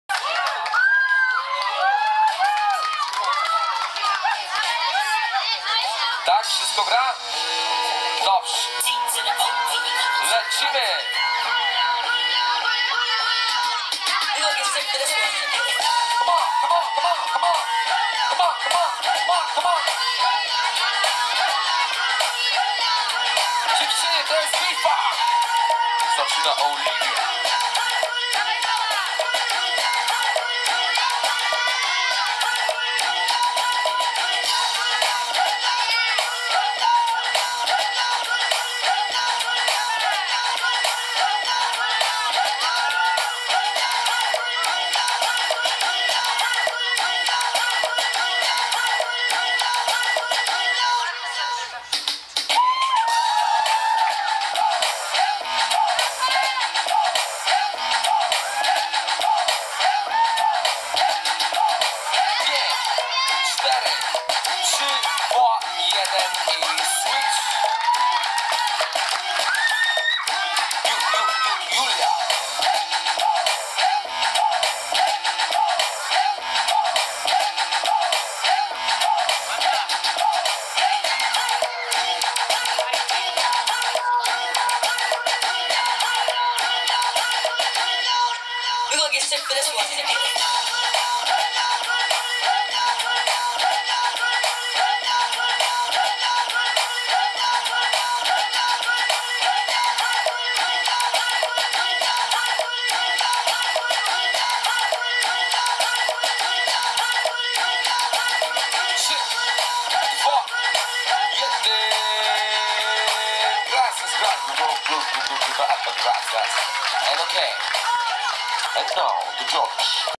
Hip Hop Kids Battle 1 vs 1 / Oliwka vs Julson
Free Mind Festival 2014 / Hip Hop Kids Battle 1 vs 1 / Oliwka vs Julson / Klub ,,FAMA'' Białystok / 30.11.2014.